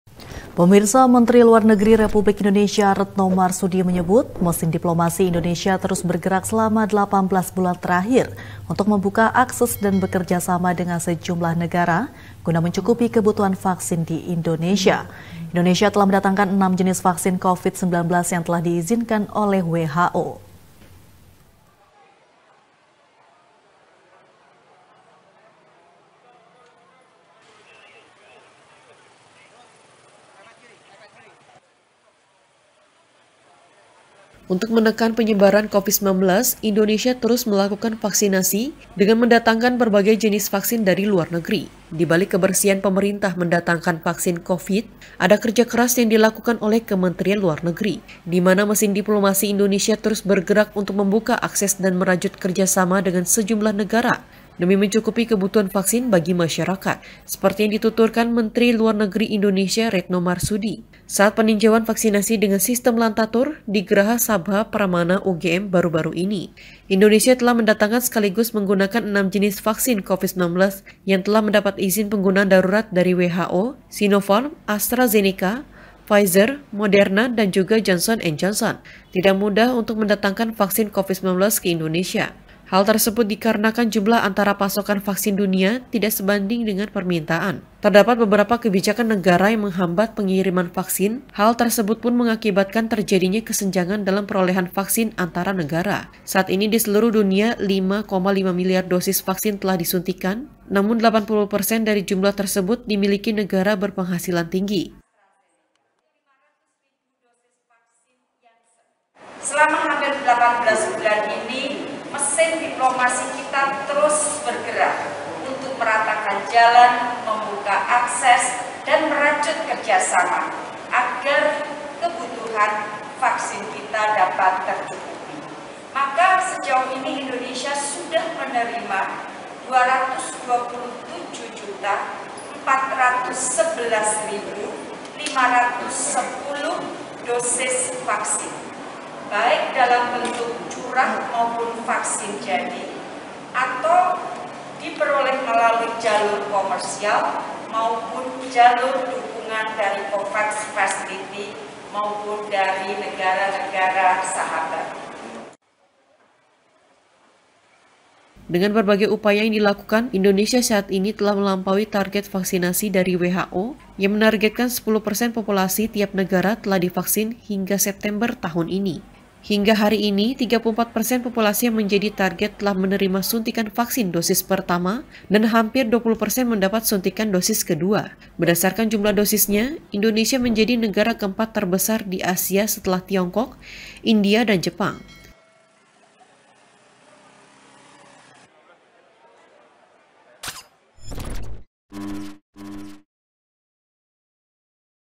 Berkat Mesin Diplomasi, Indonesia Berhasil Lampaui Target Vaksinasi dari WHO!
- Untuk menekan penyebaran Covid-19, Indonesia terus melakukan vaksinasi dengan mendatangkan berbagai jenis vaksin dari luar negeri. Dibalik keberhasilan pemerintah mendatangkan vaksin Covid, ada kerja keras yang dilakukan oleh kementerian luar negeri. Dimana mesin diplomasi Indonesia terus bergerak untuk membuka akses dan merajut kerja sama dengan sejumlah negara, demi mencukupi kebutuhan vaksin bagi masyarakat.